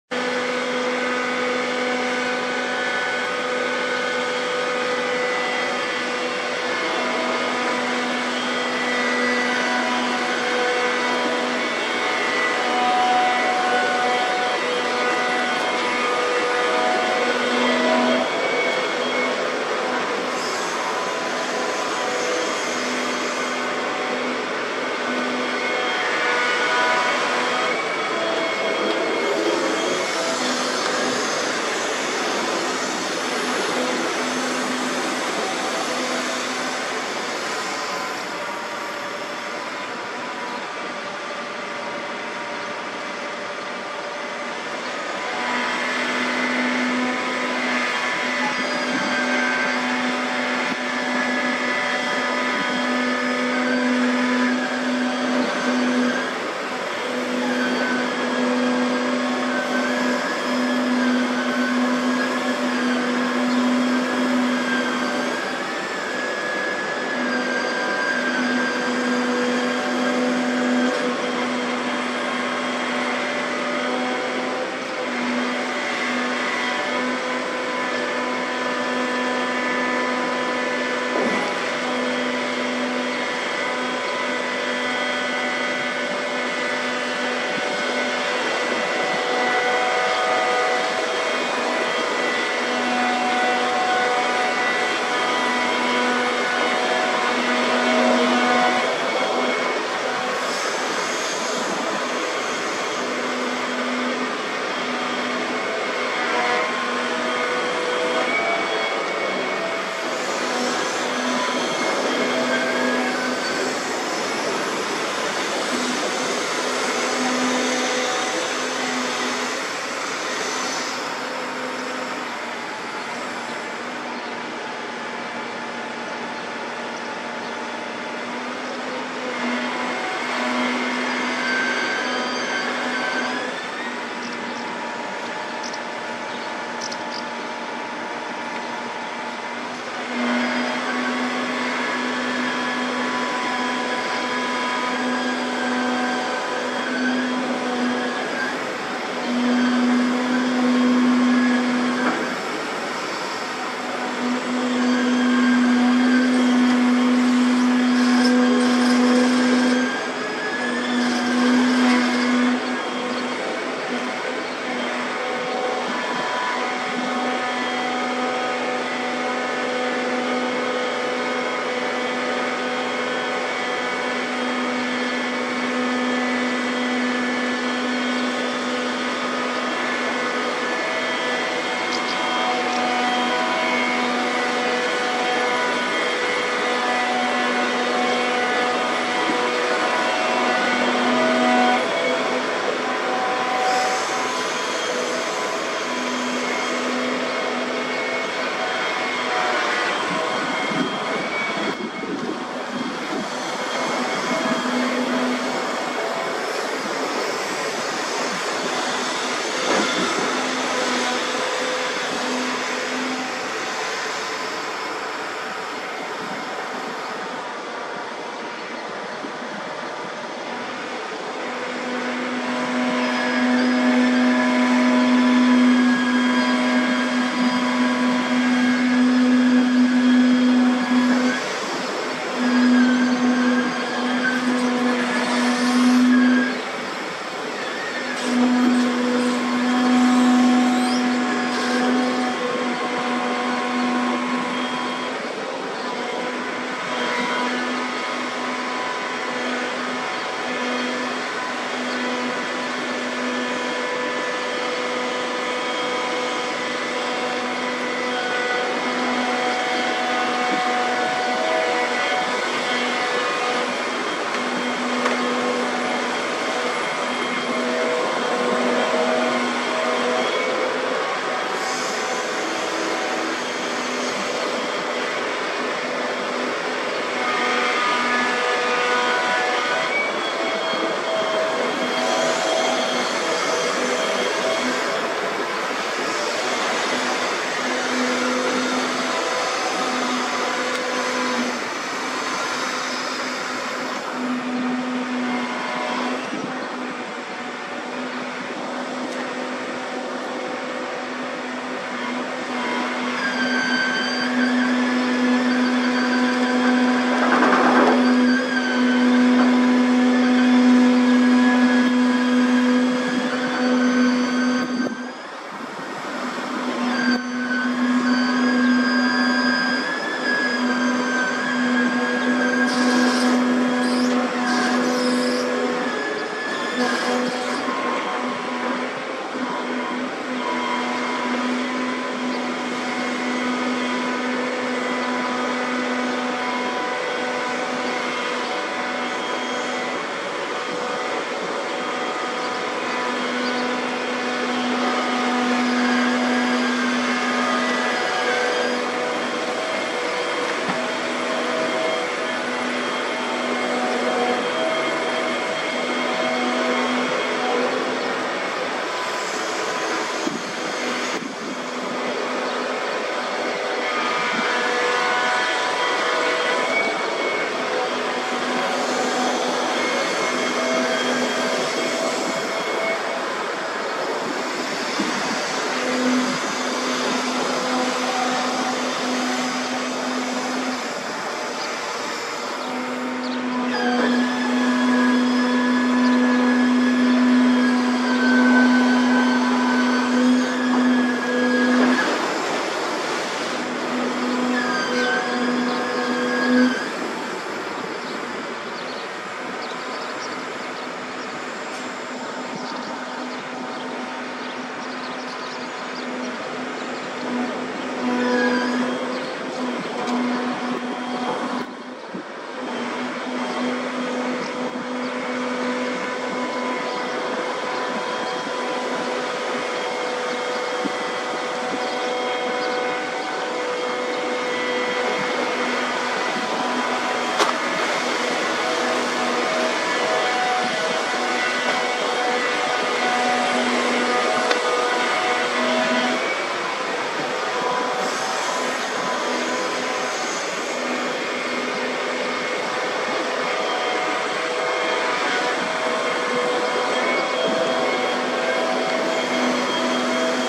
Przekop 4etap. 7. 6. Pogłębianie rzeki Elbląg widok z extra bliska.
relacja z godziny 10